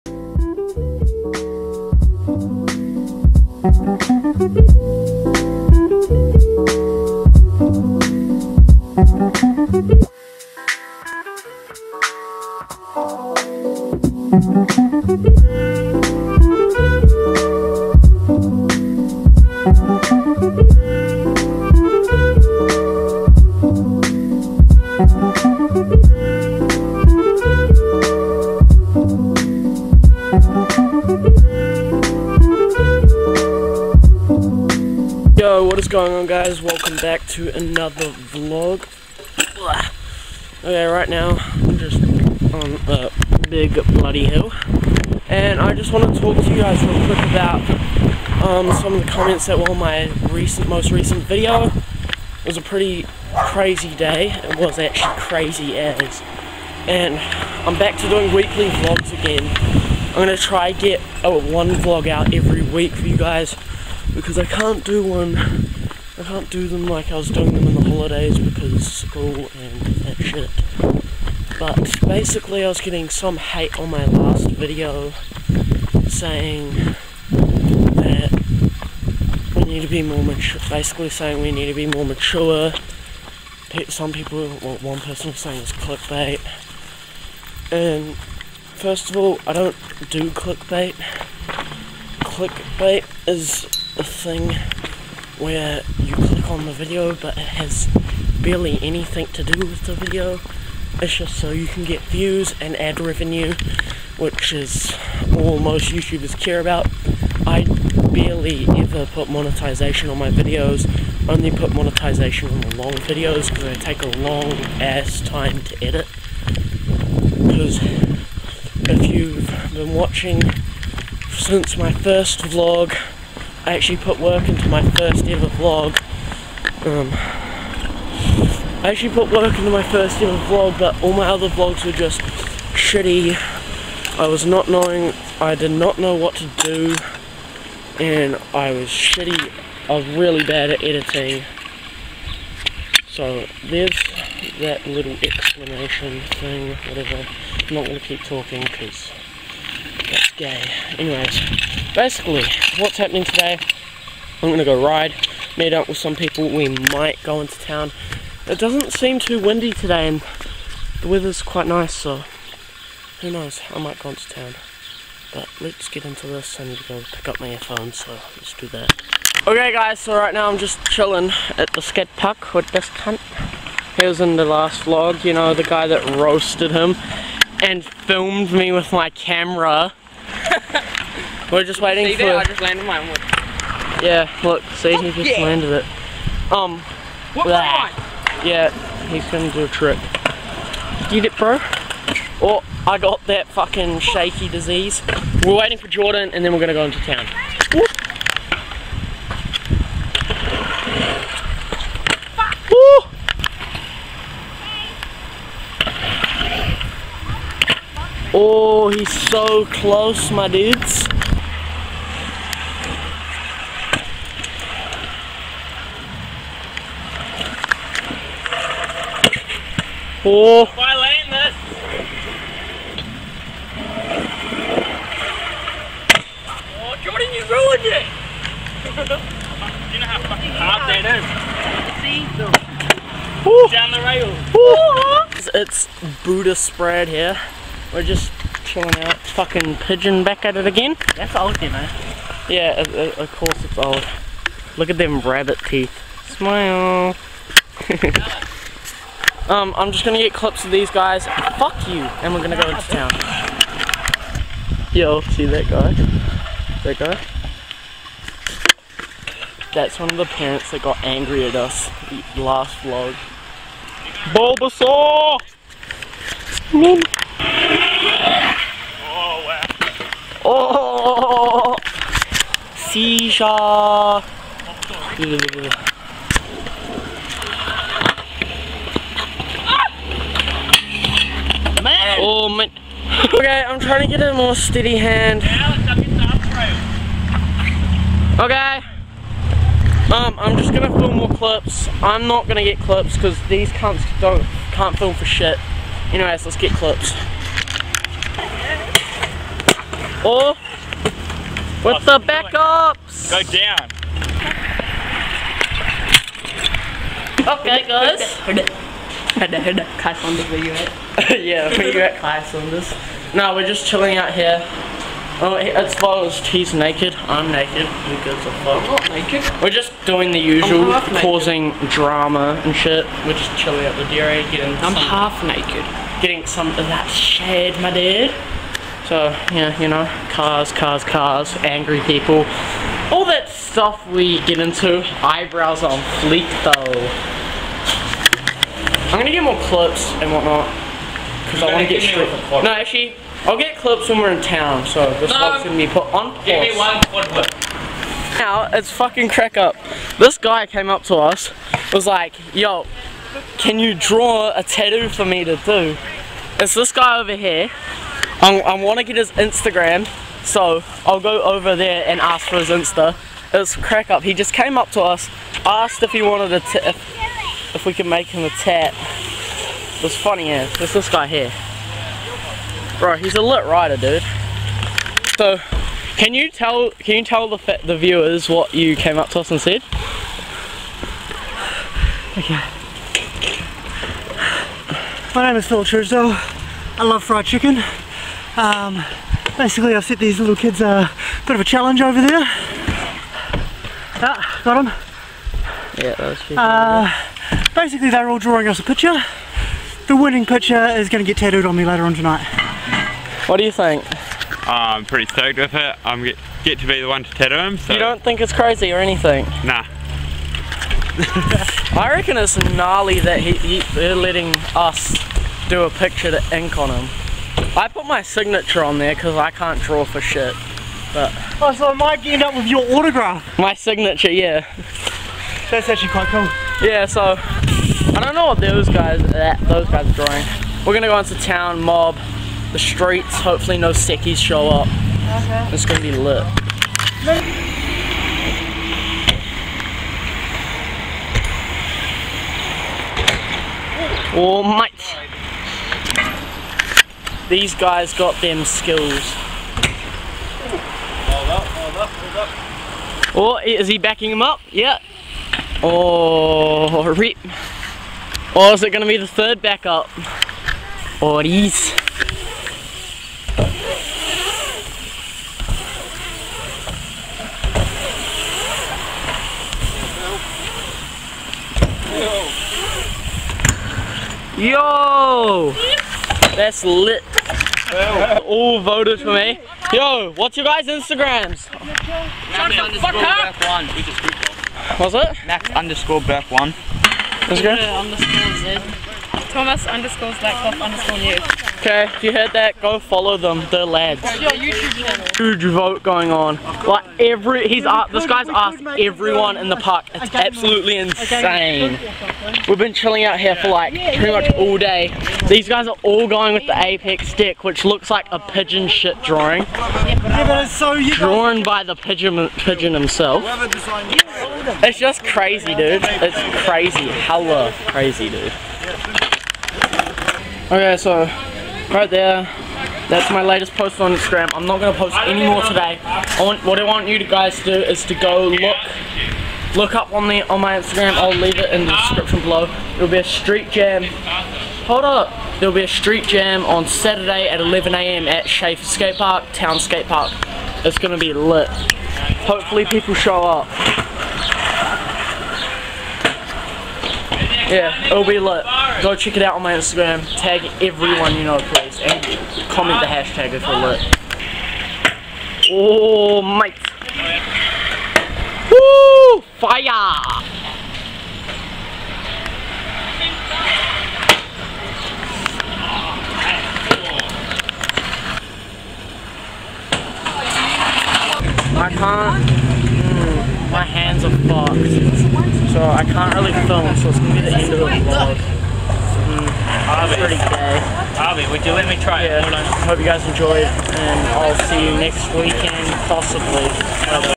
THIS GUY...(street jam coming up info in description) RAW VLOG#011
Street Jam info  okay  im hosting a street jam i will be vloging it. the street jam  will be on the 18th/saturday at town sk8  but if its raining the street jam will be put off. hope ya come along